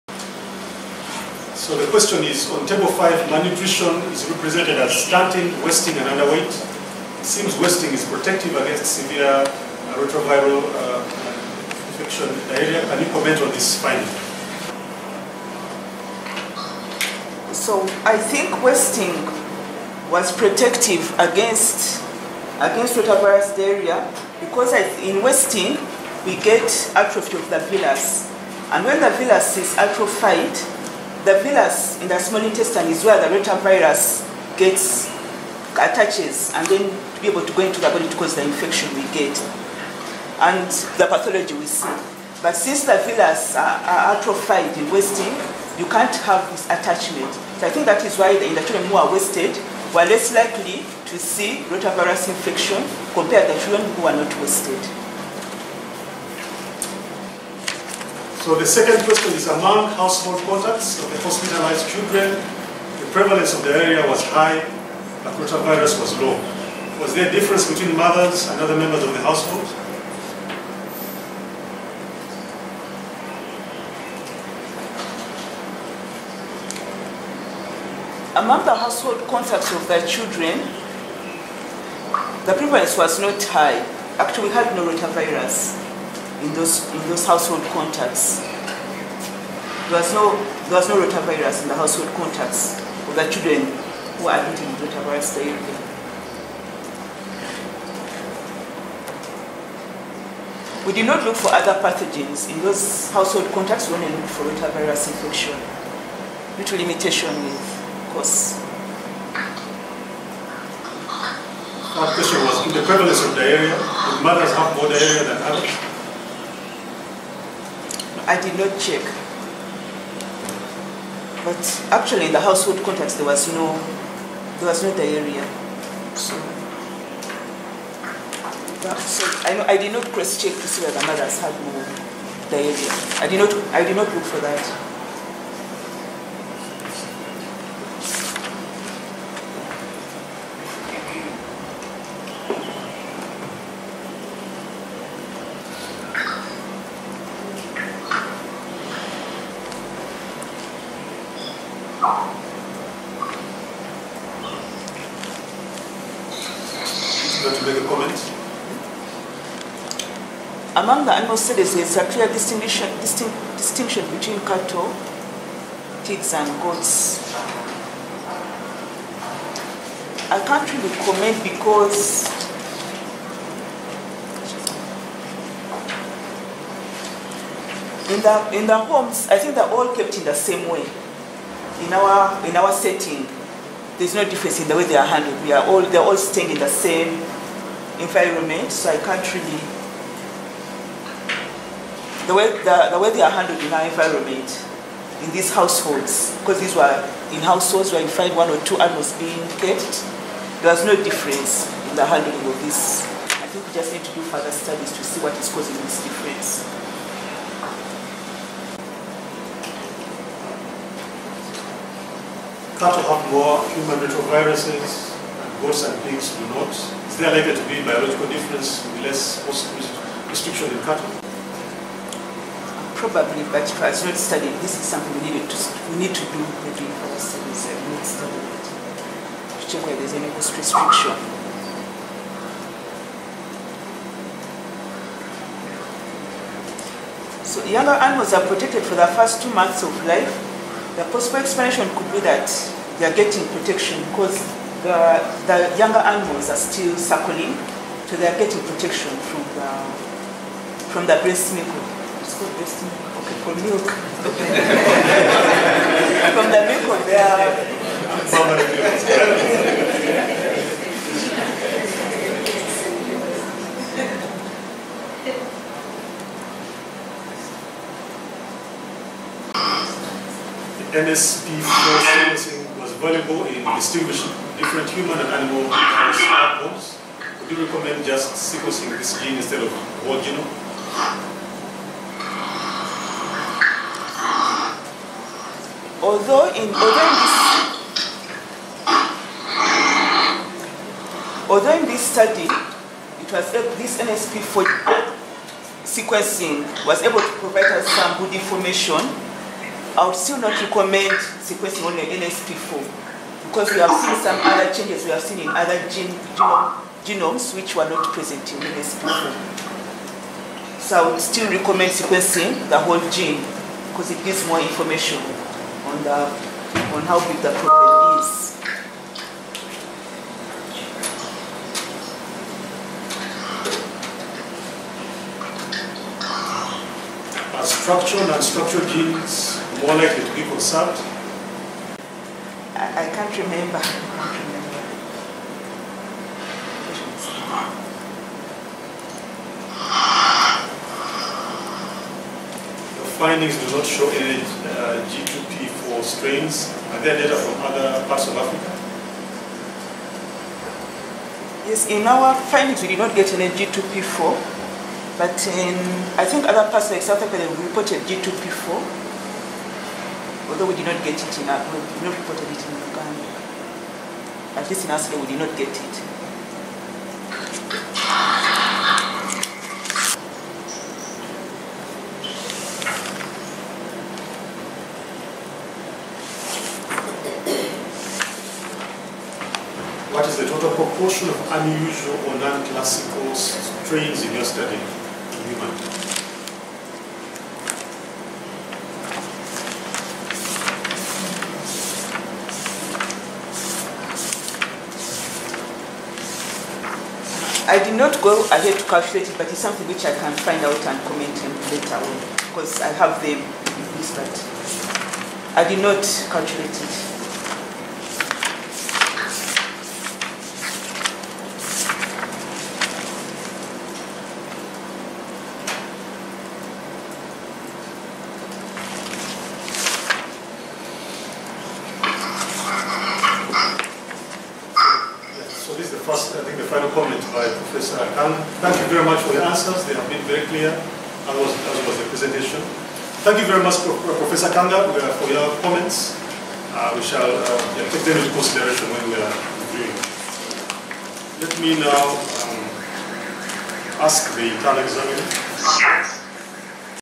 Dr. Josephine Bwogi: PhD Defence
Thesis Defense.
Title: Rotaviruses in Uganda: Molecular Epidemiology and potential of Zoonotic Transmission.